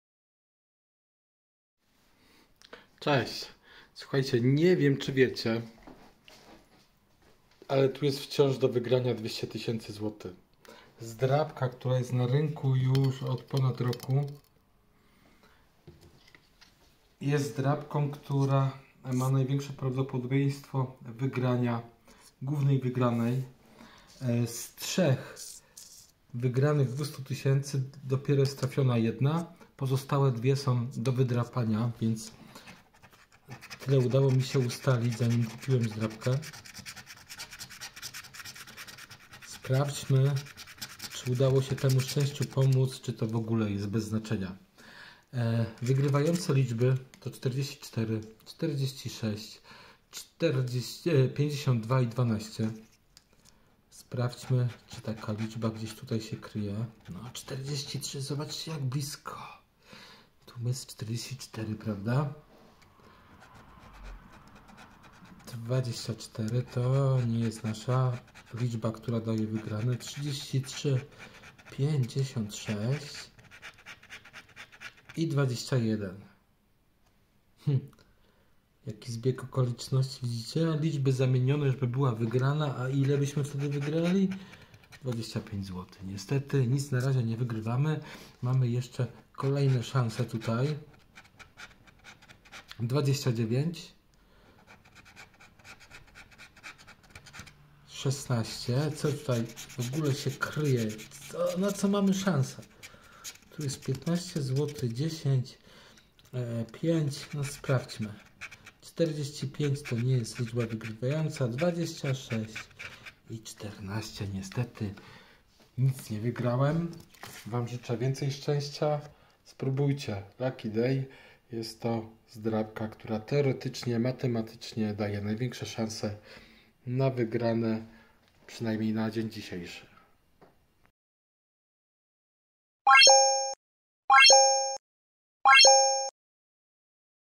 jakie zdrapki najlepiej kupić - do zgarnięcia 200 000 zł
Największe prawdopodobieństwo wygranej. Zdrapki w sprzedaży już od ponad roku a wciąż do wygrania dwie z trzech głównych nagród w wysokości 200 000 zł każda.